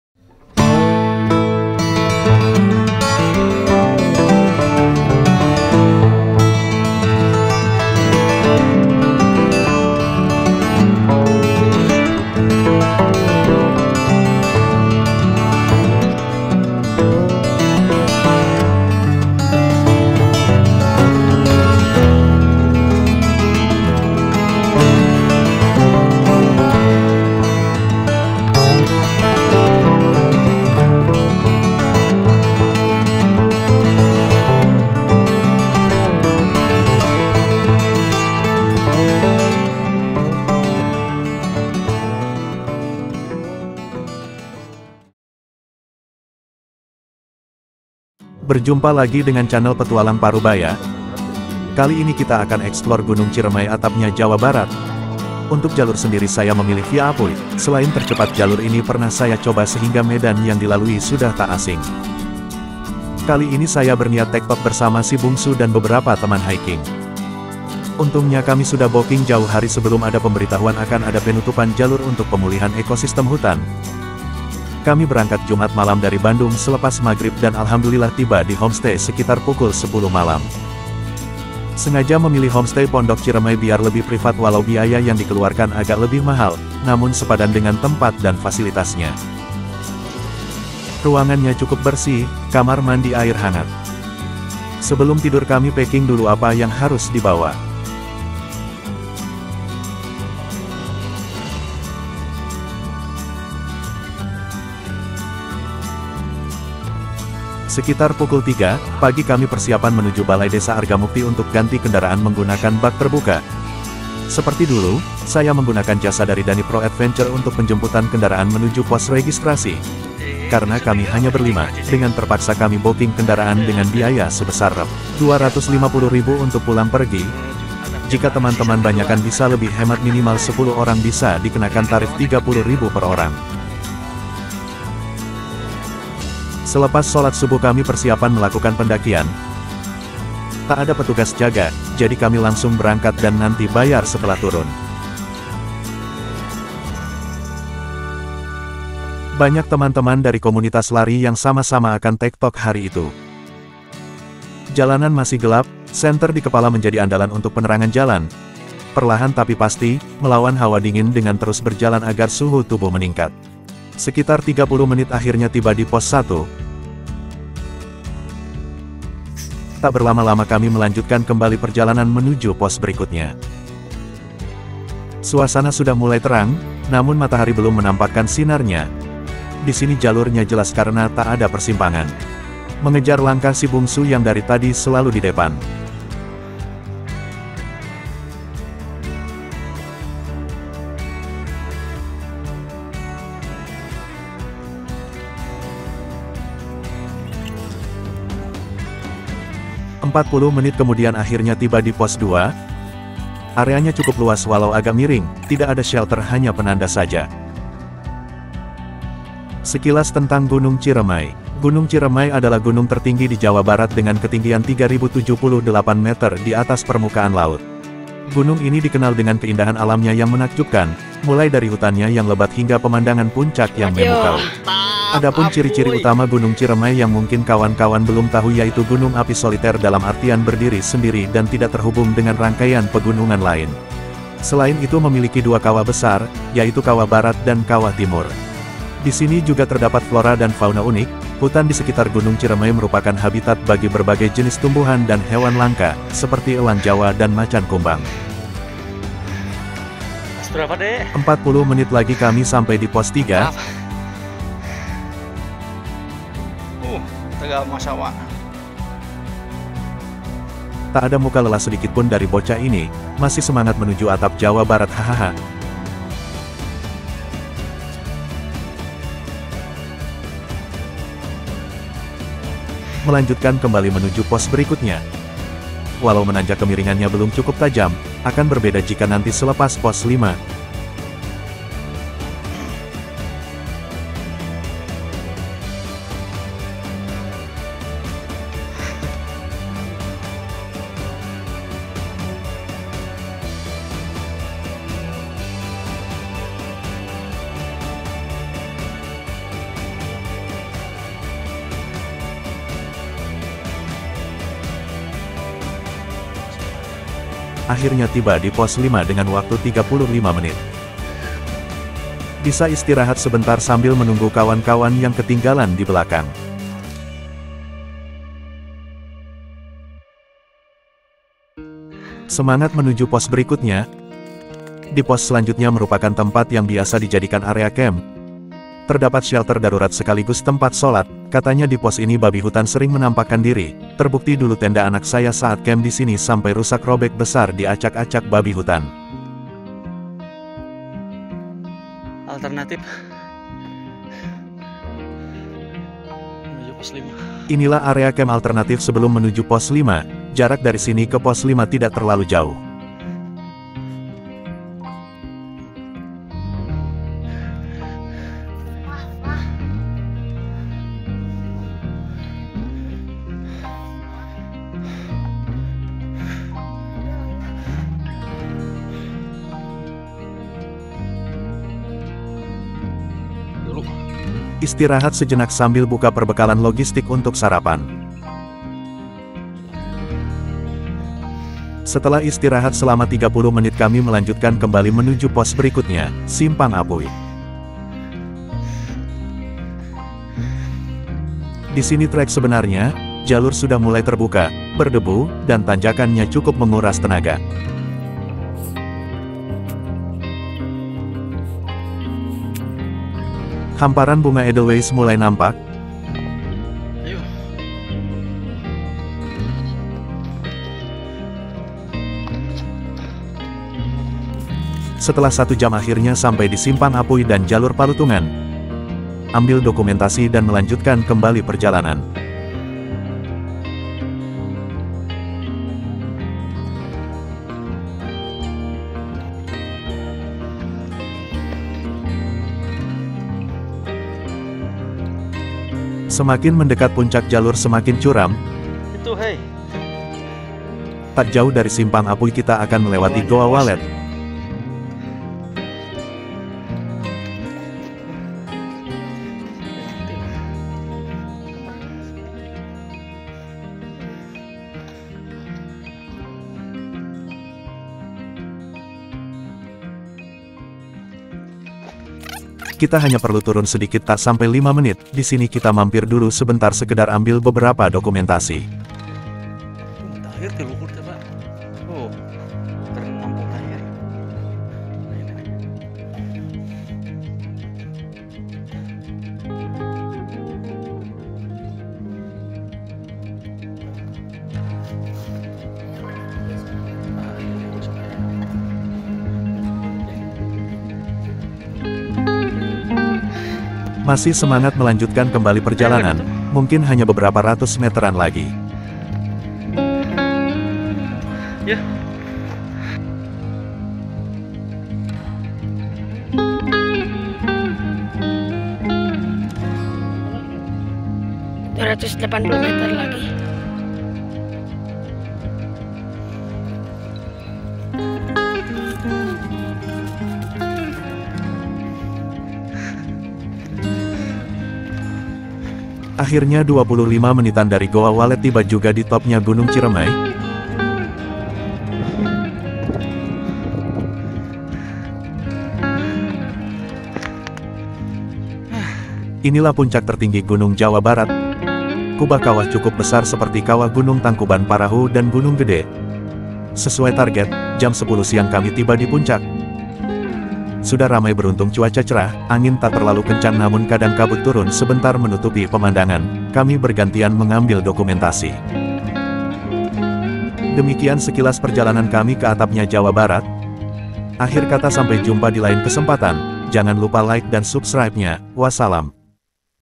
GUNUNG CIREMAI VIA APUY TERBARU SEBELUM PENUTUPAN, FREE MAP GPX
Pos 5 istirahat 30 menit

Pos 5 - Simpang Apuy 1 jam
Simpang Apuy - Top 30 menit

Rp. 77.500 On the Spot, tektok (pulang pergi) dengan kemping harga sama

Jasa sewa Kolt bak borongan 250 rb - 300 rb
(Kalau mau murah gabung ramai ramai minimal 10 orang bisa di kenakan biaya Rp 30.000,- pulang pergi, kecuali membawa motor bisa langsung parkir di Pos registrasi parkir cuma bayar Rp. 5.000)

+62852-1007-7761